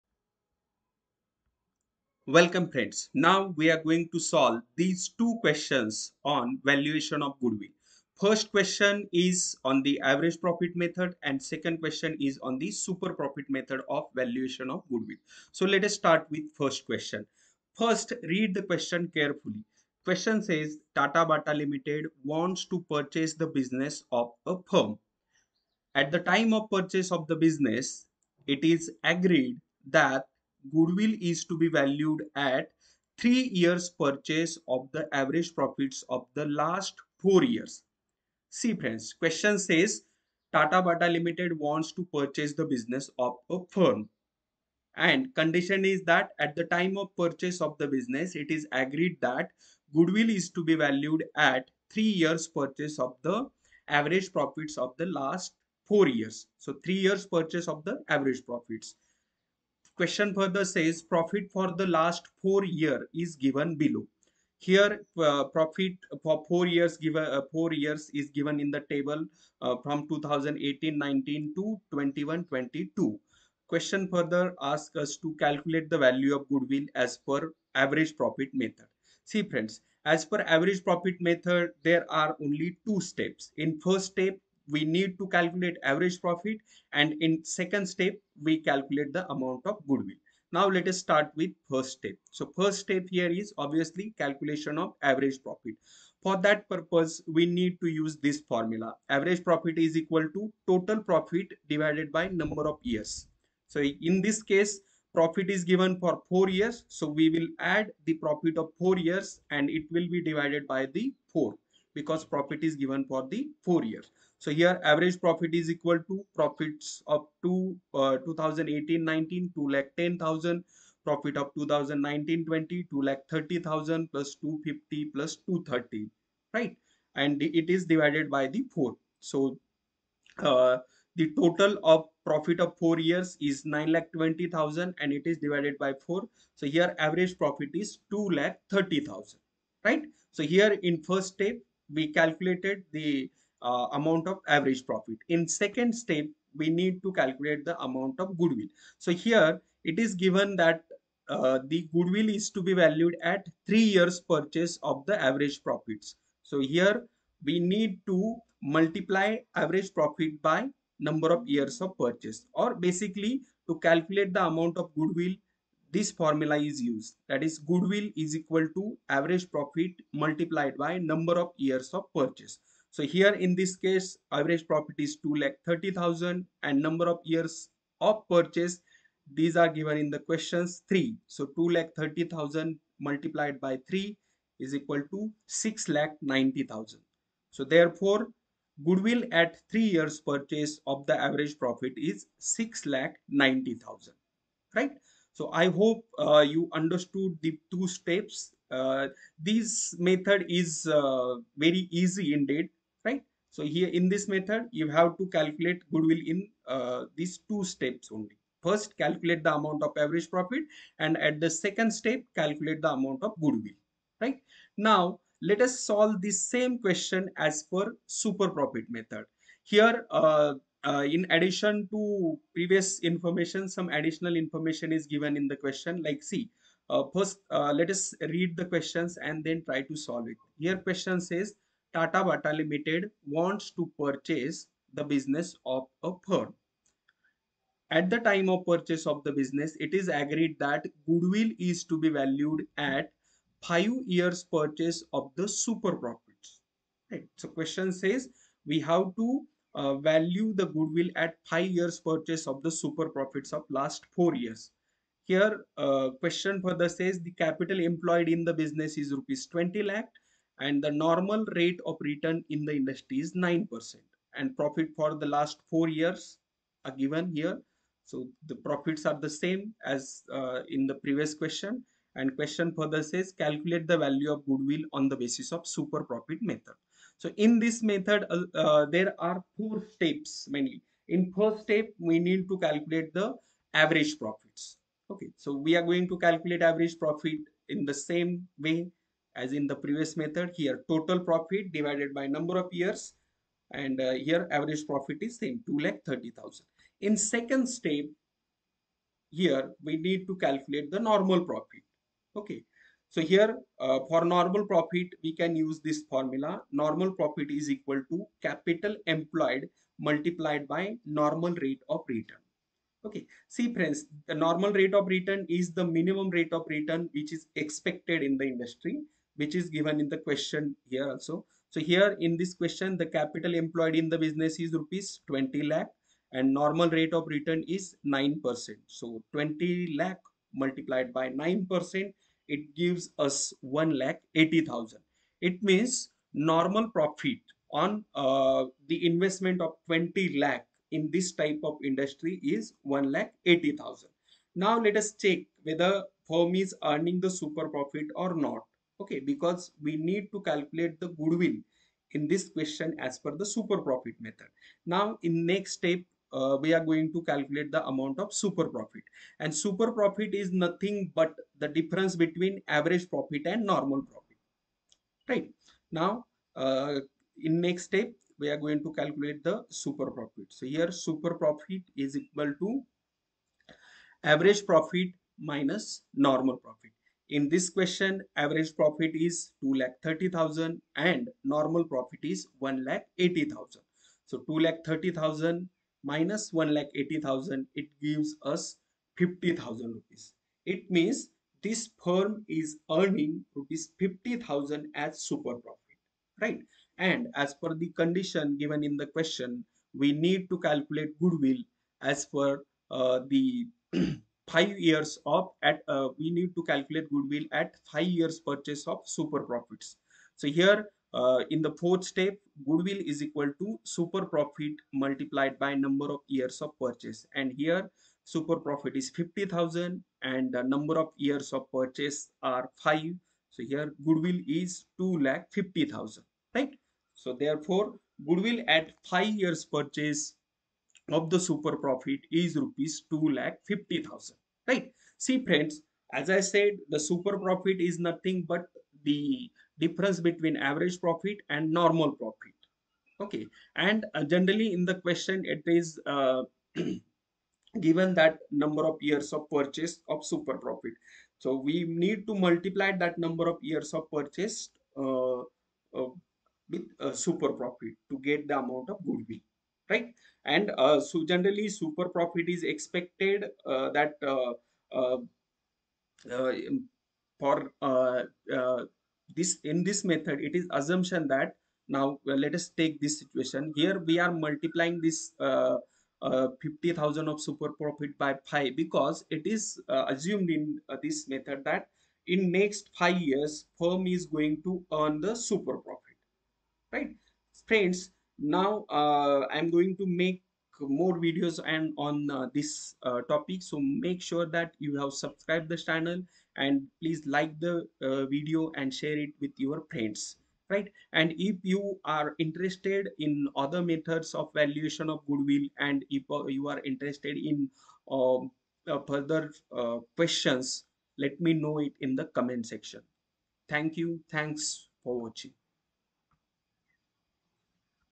Valuation Of Goodwill Average Profit Method Super Profit Method
The Video explains the steps in solution of question on valuation of goodwill as per average profit Method super profit method